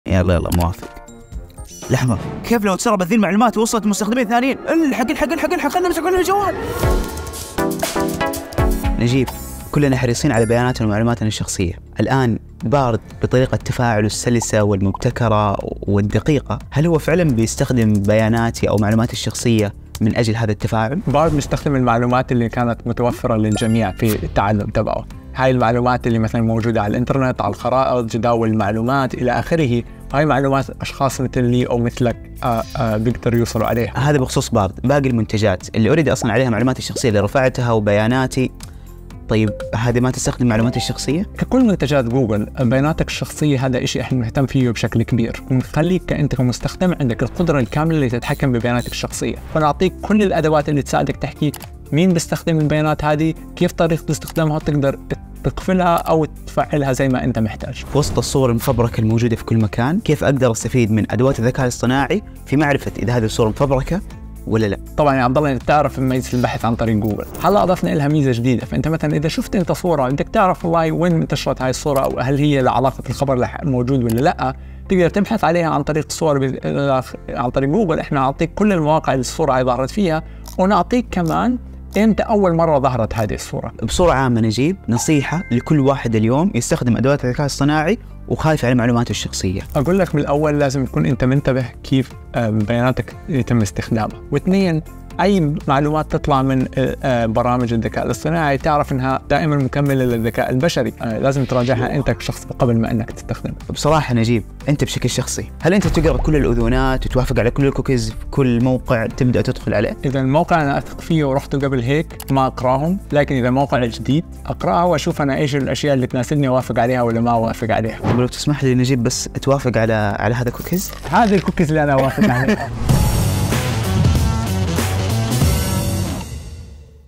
هل بياناتك بأمان مع أدوات الذكاء الاصطناعي؟
هل تقنيات الذكاء الاصطناعي آمنة؟ تعرف على كيفية الحفاظ على خصوصيتك على الإنترنت.
رمضان قادم، كيف ممكن أحمي بياناتي على الإنترنت وخاصة لما أستخدم تقنيات جديدة؟